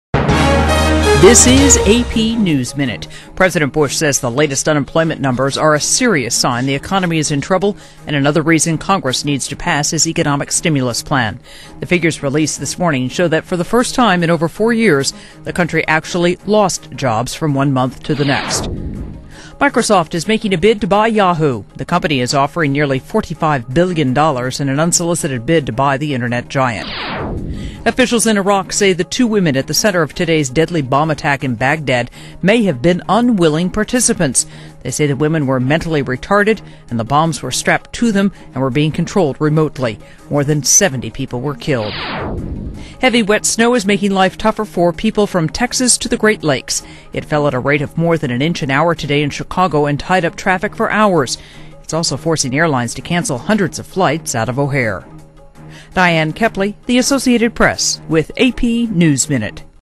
AP Top Stories
Here's the latest news for Friday, Feb. 1: Economy loses jobs; Microsoft makes bid for Yahoo; Blast kills 70 in Iraq; Snow blankets Midwest.